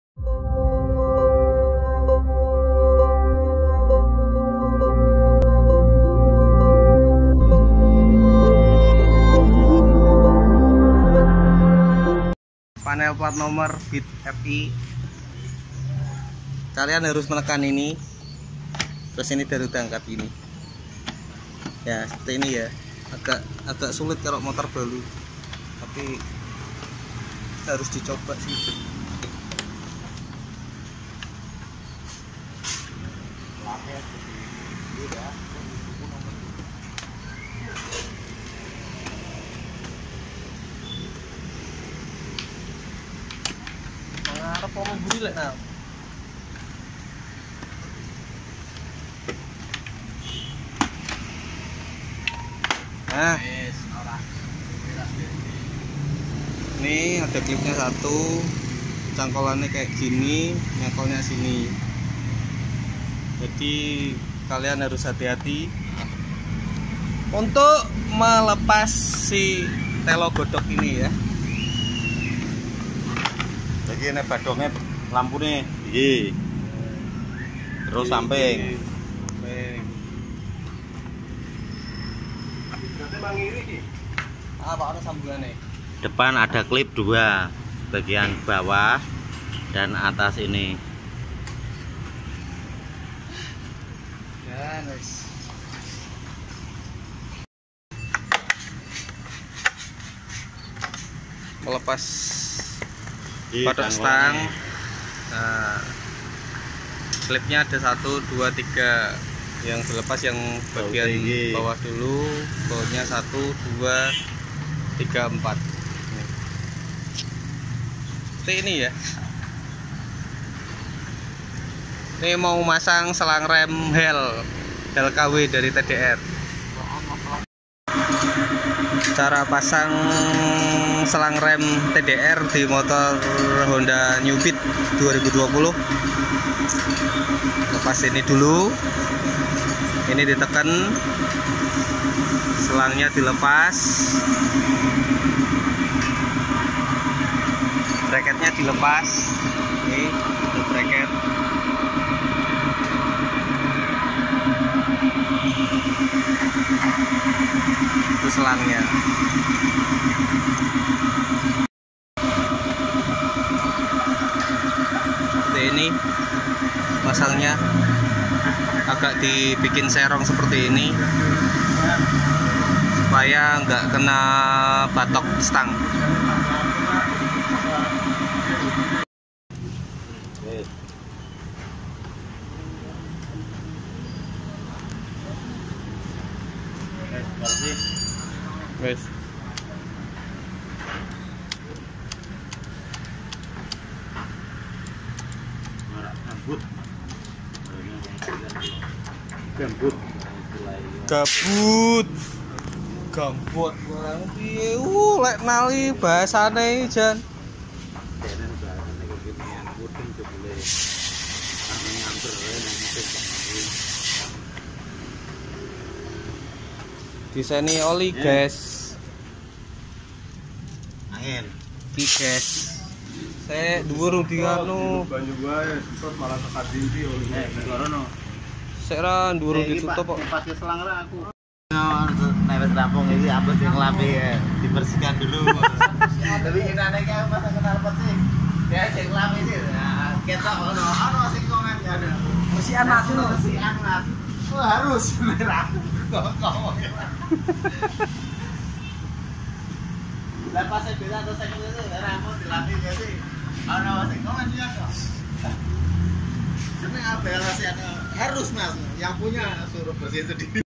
CARA MELEPAS PANEL(dasi), BATOK STANG, LANJUT PASANG SELANG REM TDR DI NEW HONDA BEAT Fi 2020
Selamat menonton semuanya